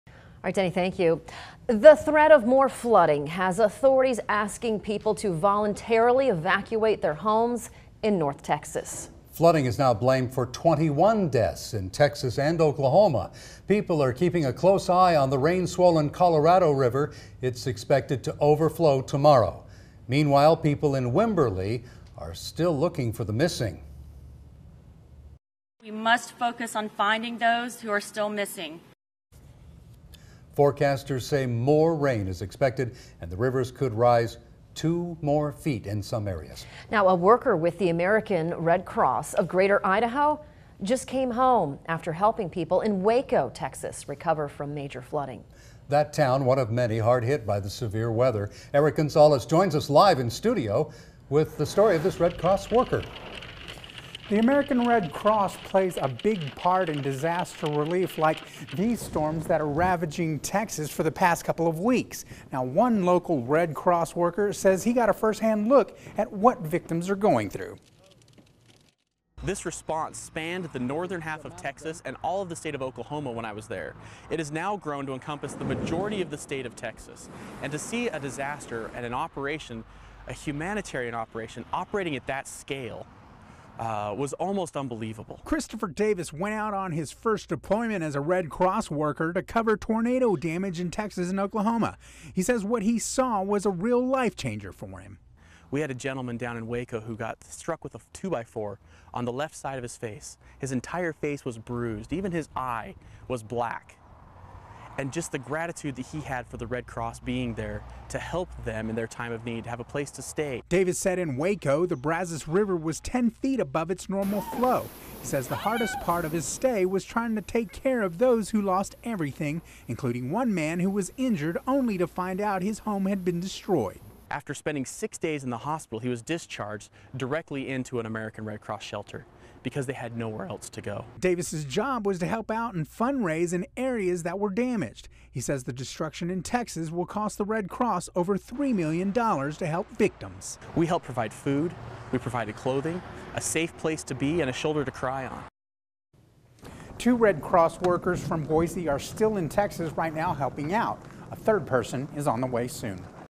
Idaho Red Cross Volunteer Shares Stories From Texas
A volunteer from Idaho went to Texas to support Red Cross efforts in the area torn apart by devastating floods.